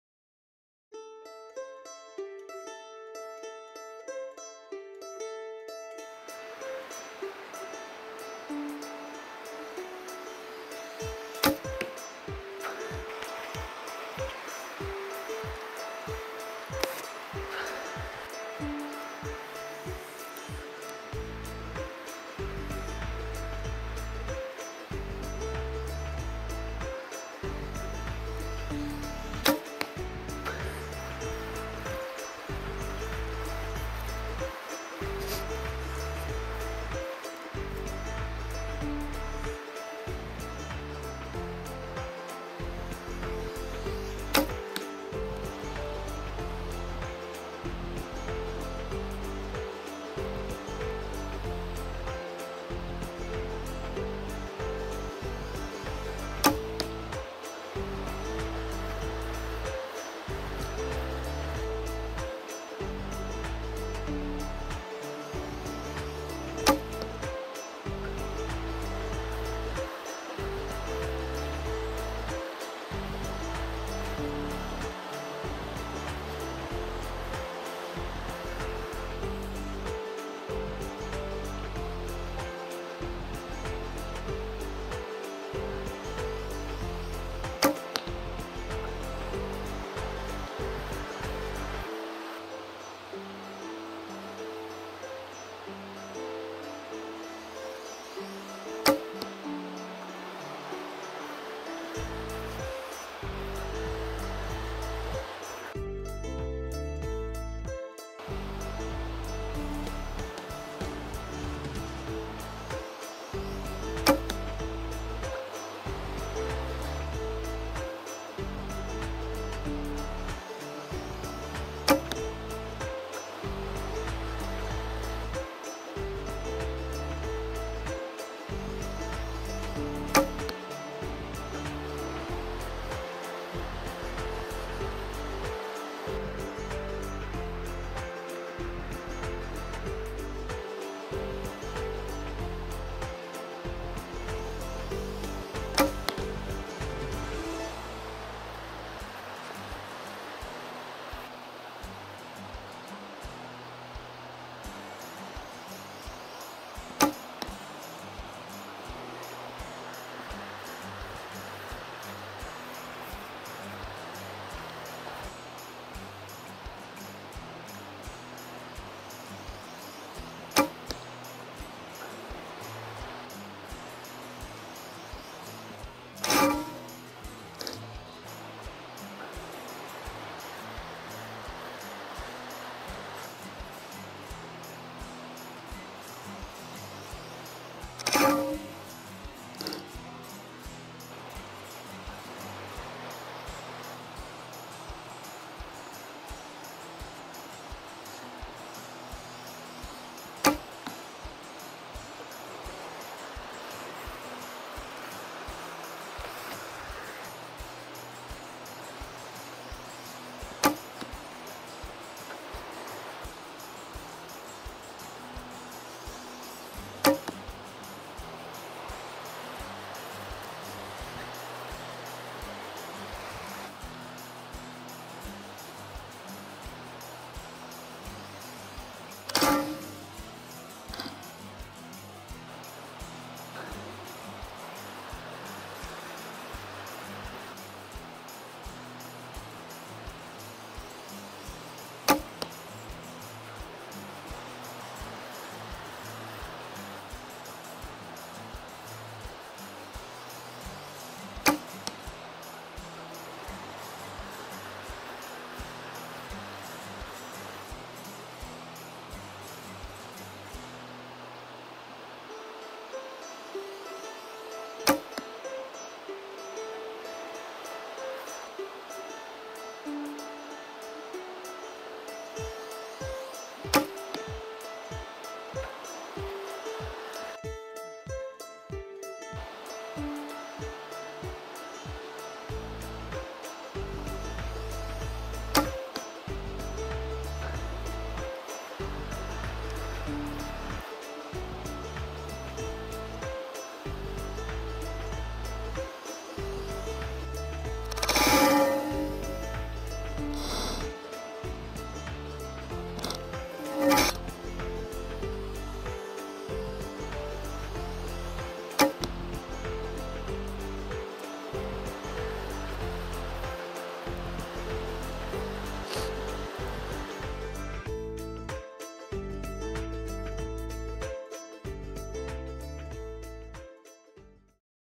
Longbow Shooting w/Sony FDR X1000V Action Cam & Lumenok's
In this video I am practice shooting with my Dwyer Endeavor Longbow  while filming with the Sony FDR X1000V action camera mounted on the head strap in the 1080 60P recording mode and the XAVC-S codec with image stabilization on. I am also shooting with green Lumenok lighted arrow noks. I am testing the setup for filming my hunts this season.